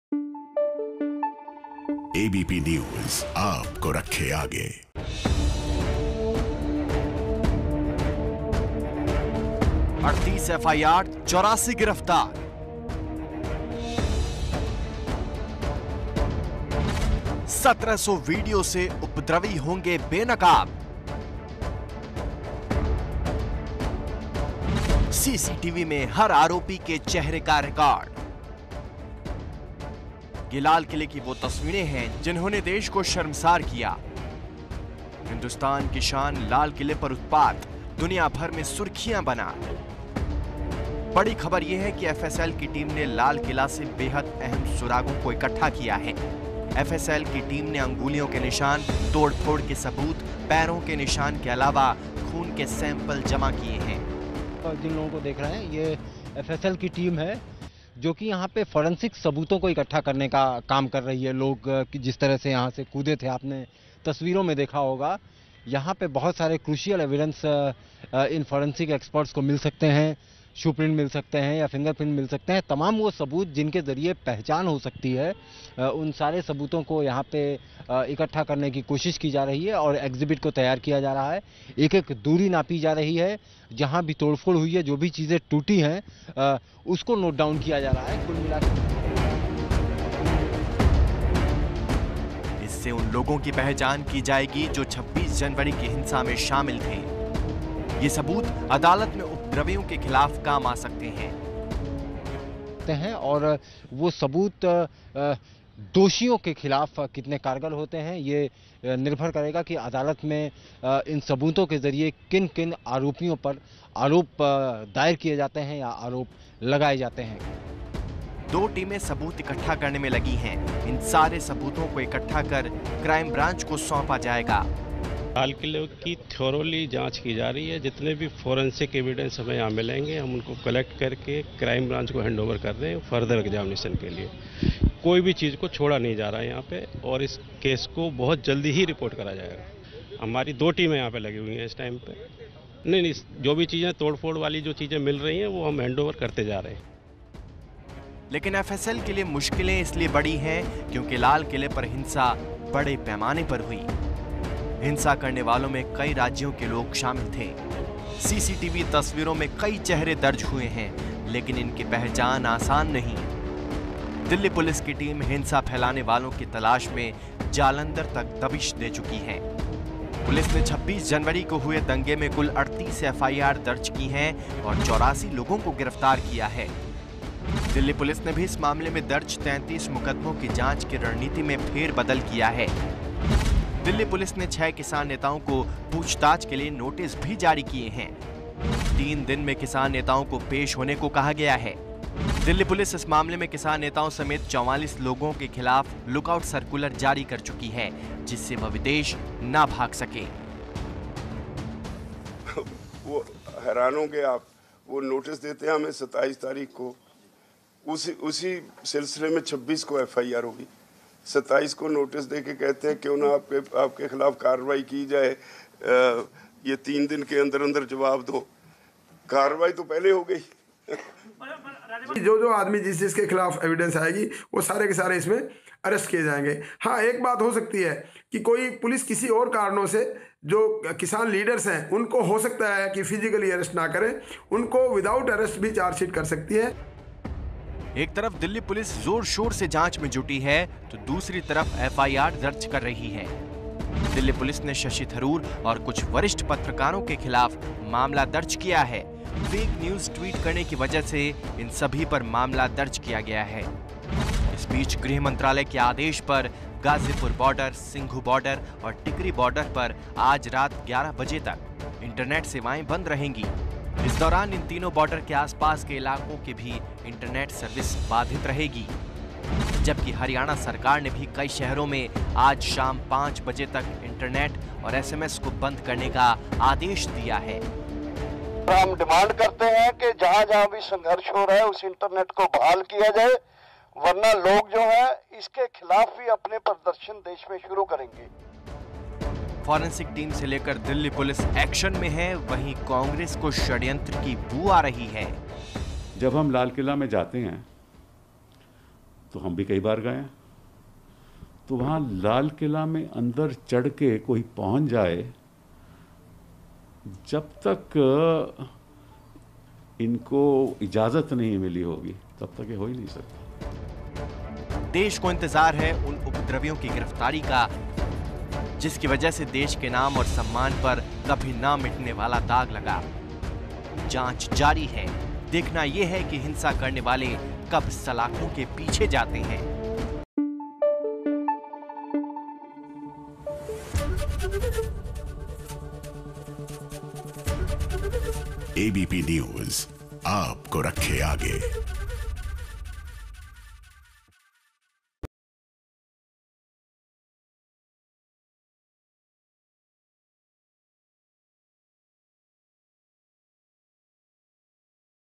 Tractor Rally : पुलिस को हिंसा के मिले 1700 वीडियो, हिंसा मामले में 38 केस दर्ज, 84 लोग गिरफ्तार
दिल्ली पुलिस ने 26 जनवरी की हिंसा के मामले में 38 एफआईआर दर्ज की है... और 84 लोगों को गिरफ्तार किया है. फॉरेंसिक टीम ने भी कल लाल किला में सबूत जुटाए. ये सारे सबूत अदालत में आरोपियों के खिलाफ अहम सबूत बन सकते हैं. और उन उपद्रवियों को सलाखों के पीछे भेज सकते हैं.